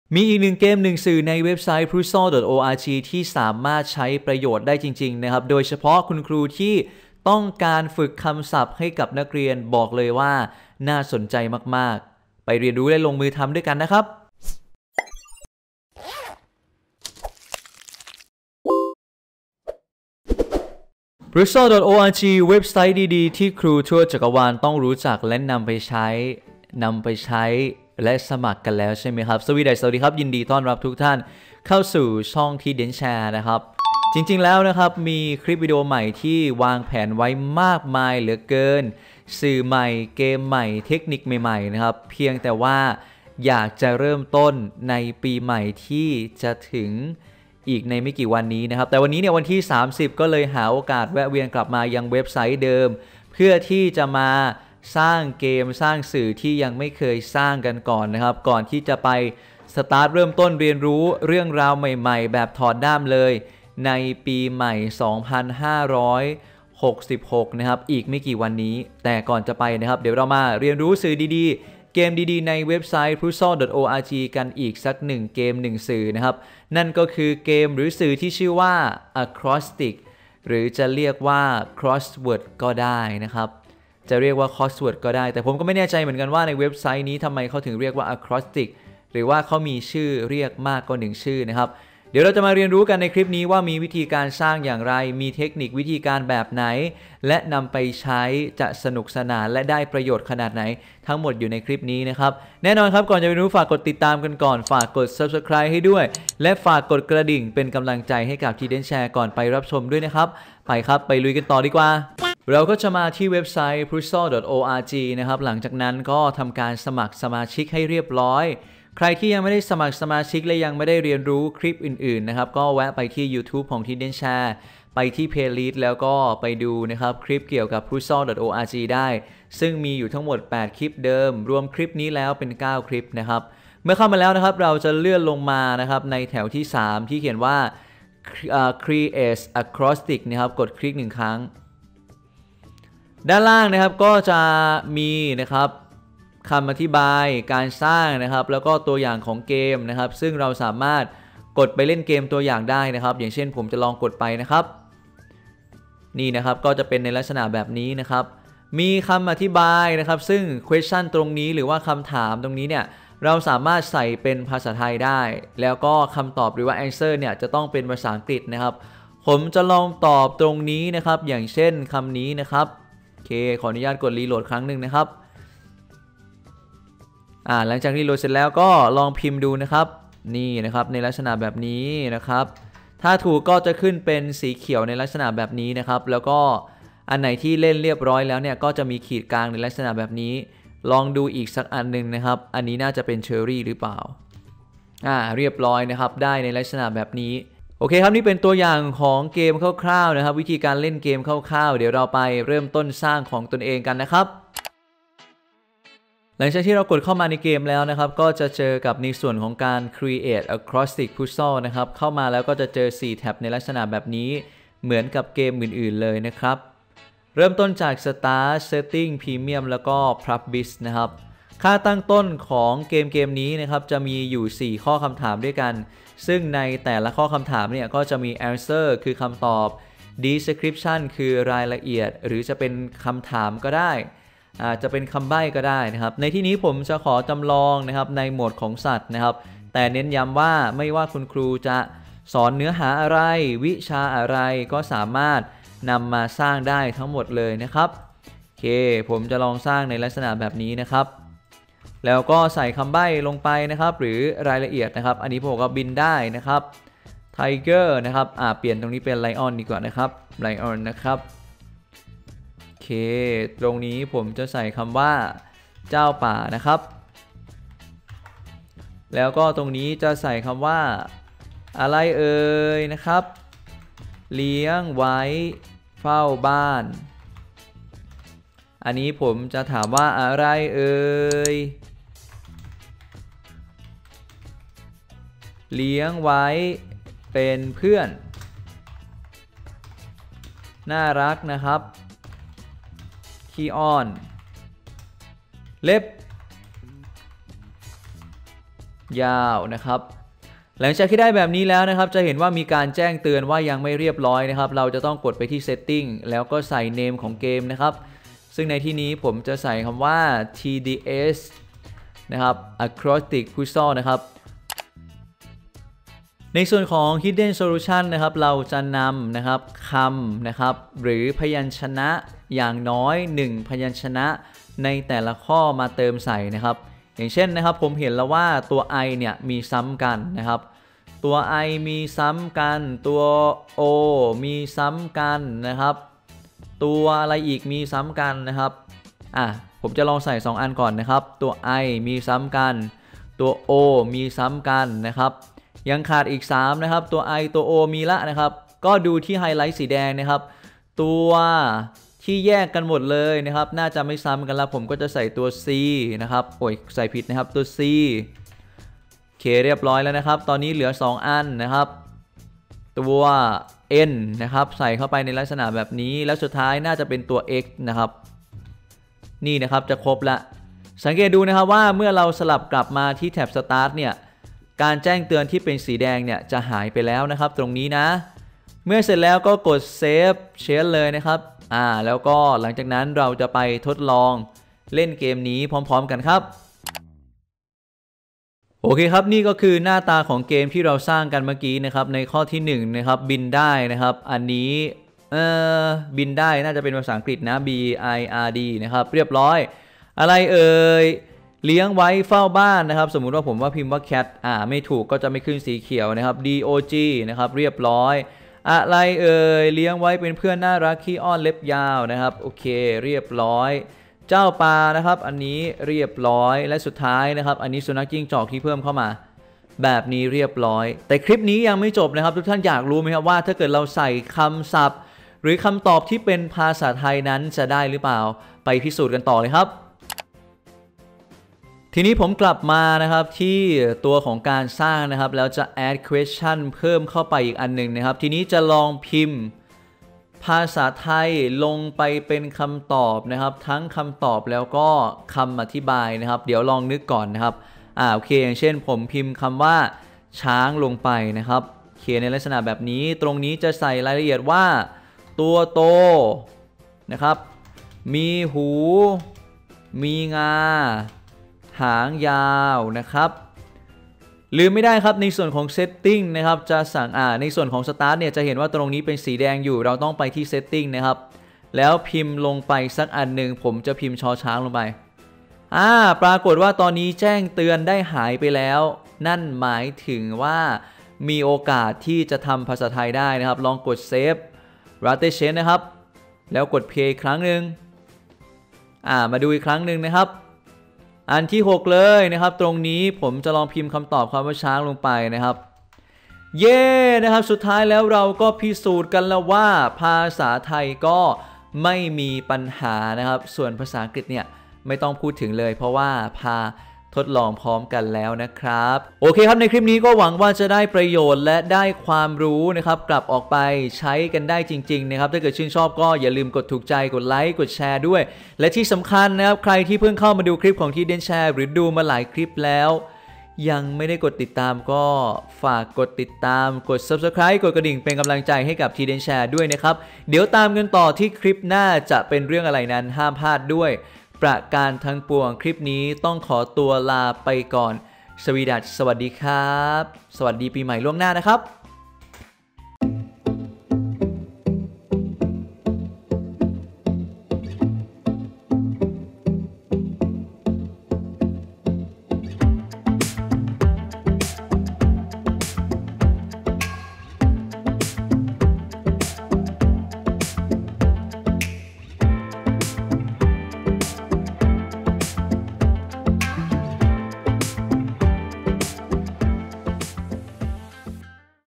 สร้างเกม Crossword ง่าย ๆ ทุกวิชา (ภาษาไทย+อังกฤษ ไม่มีปัญหา) | puzzel EP.9
คลิปวิดีโอนี้ : คลิปวิดีโอนี้ทุกท่านจะได้เรียนรู้การสร้างเกม Crossword ง่าย ๆ จะง่ายจริงหรือไม่ ไปเรียนรู้กันครับ